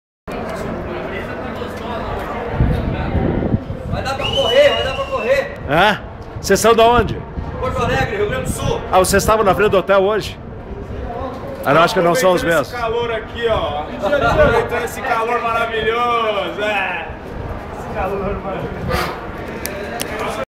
Torcedores do Brasil chegam ao estádio Ellis Park para conferir Brasil e Coreia do Norte